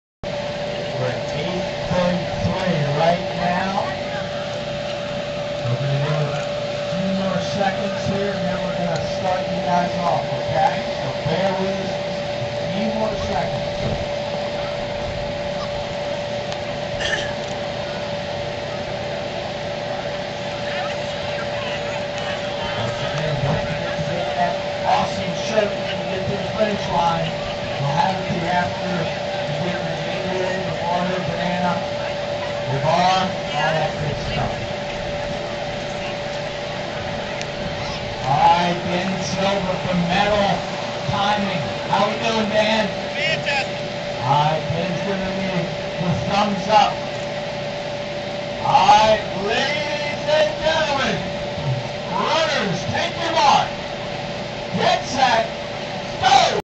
St. Charles 5k start 2012 part 1.MOV
Runners at the starting line for the the 2012 St. Charles Running Festival 5K, April 14, 2012.